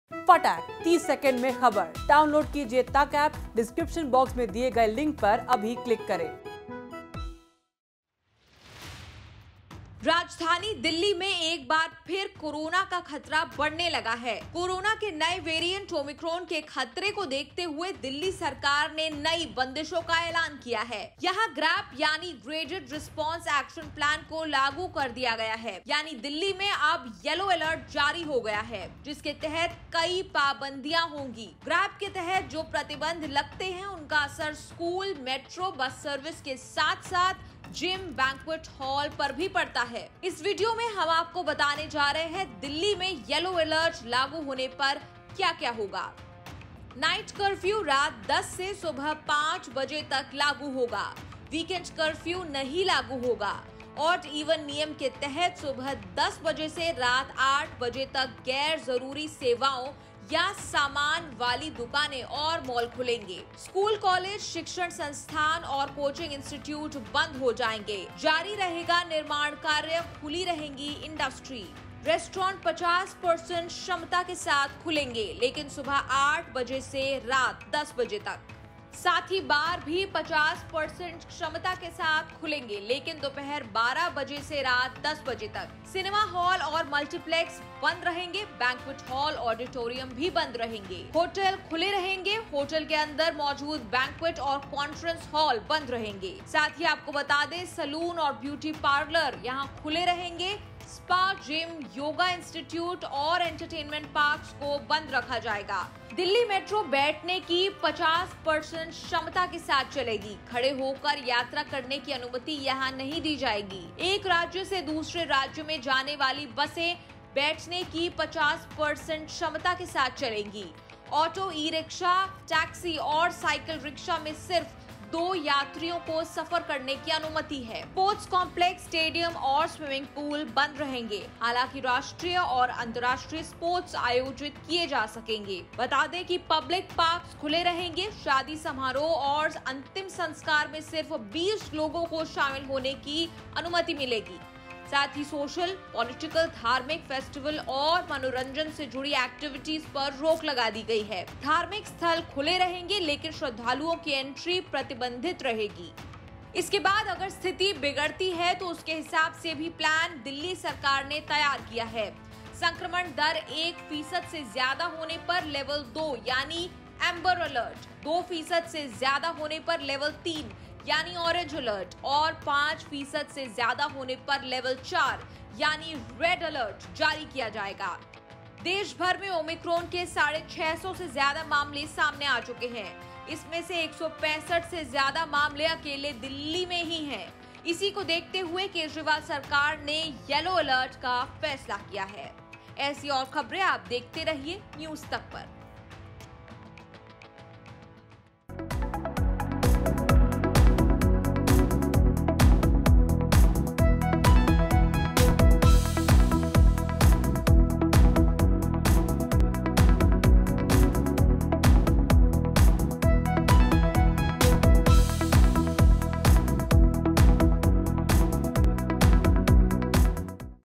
Omicron: सरकार ने किया नई पाबंदियों का ऐलान, क्या बंद क्या खुला?
राजधानी दिल्ली में एक बार फिर कोरोना का खतरा बढ़ने लगा है... कोरोना के नए वैरिएंट ओमिक्रॉन के खतरे को देखते हुए दिल्ली सरकार ने नई बंदिशों का ऐलान किया है... यहां GRAP यानी ग्रेडेड रिस्पांस एक्शन प्लान को लागू कर दिया गया है... यानी दिल्ली में अब येलो अलर्ट जारी हो गया है.. देखें वीडियो...